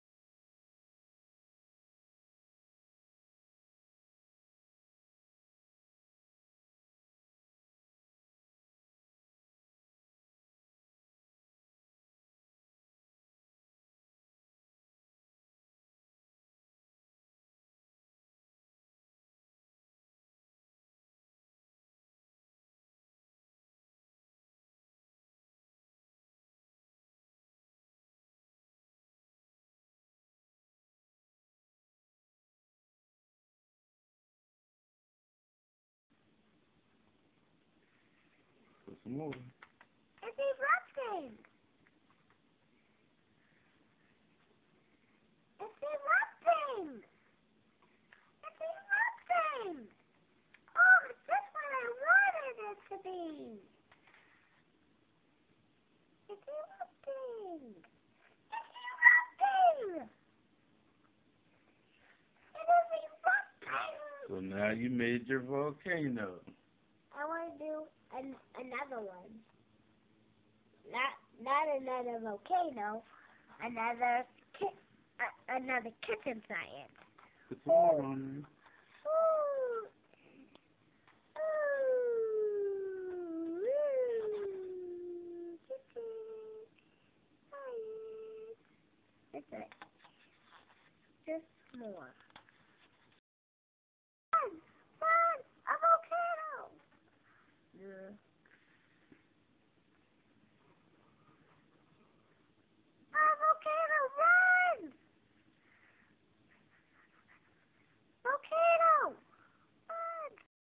SJvolcano
Sj's kitchen experiment: His Volcano. With vinegar , baking soda, and liquid soap.